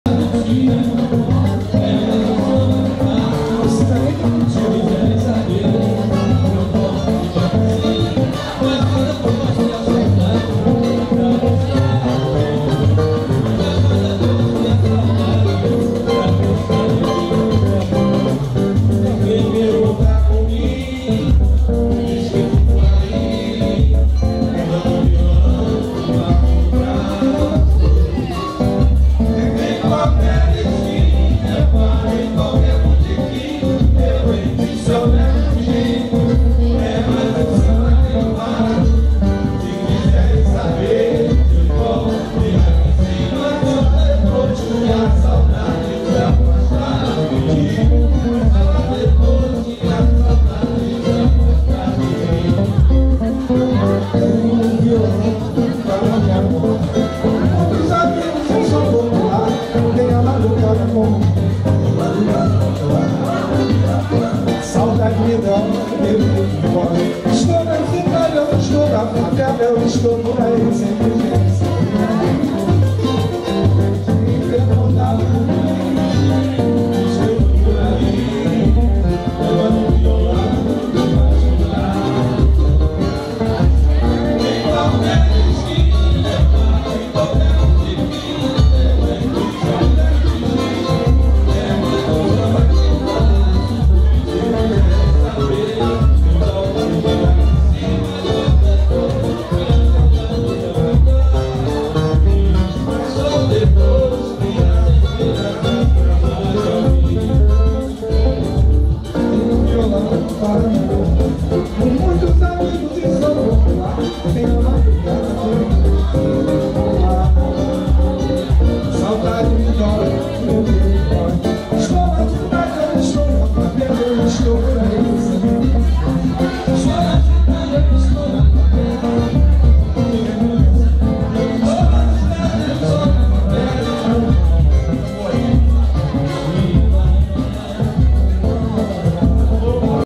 EDUARDO GALOTTI TRAPICHE GAMBOA SAMBA CARIOCA
EDUARDO GALOTTI e grupo de samba no TRAPICHE GAMBOA um dos melhores lugares para curtir o bom samba carioca, estive lá e provei.